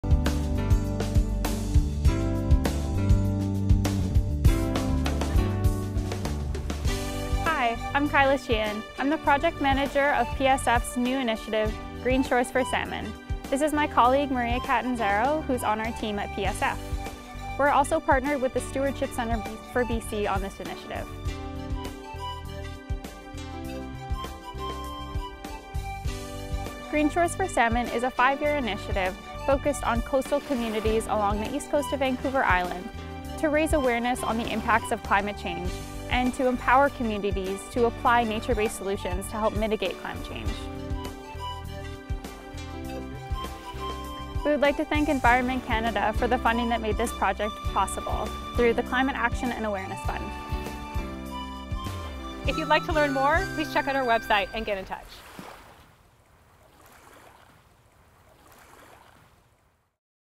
Learn about the Resilient Coasts for Salmon Program
Traditional approaches to protecting coastal communities from ocean waves and erosion have involved hardening shorelines with structures like dykes, seawalls and jetties. However, these artificial structures may cause more problems than they solve. The Green Shores for Salmon Program will provide education and training to the public and shoreline professionals which we hope will result in greater adoption of nature-based solutions on Vancouver Island’s East Coast. The goal is to increase the naturalization and greening of shorelines, with associated benefits of reduced flood risk and erosion, retention of forage fish habitat, and promotion of healthy shoreline ecosystems on high value salmon habitat.